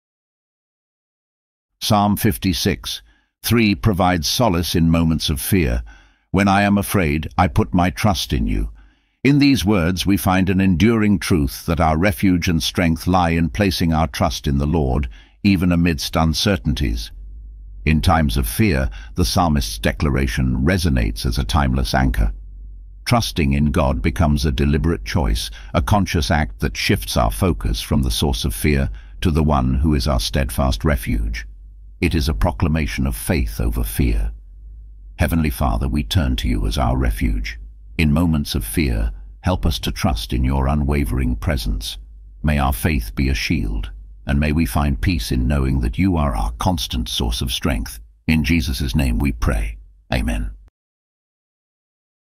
Trust in Times of Fear - Psalm 56:3
Trust in Times of Fear

Psalm 56:3 provides solace in moments of fear: "When I am afraid, I put my trust in you." In these words, we find an enduring truth that our refuge and strength lie in placing our trust in the Lord, even amidst uncertainties.

In times of fear, the psalmist's declaration resonates as a timeless anchor. Trusting in God becomes a deliberate choice, a conscious act that shifts our focus from the source of fear to the One who is our steadfast refuge. It is a proclamation of faith over fear.

Heavenly Father, we turn to you as our refuge. In moments of fear, help us to trust in your unwavering presence. May our faith be a shield, and may we find peace in knowing that you are our constant source of strength.

In Jesus' name, we pray.

Amen.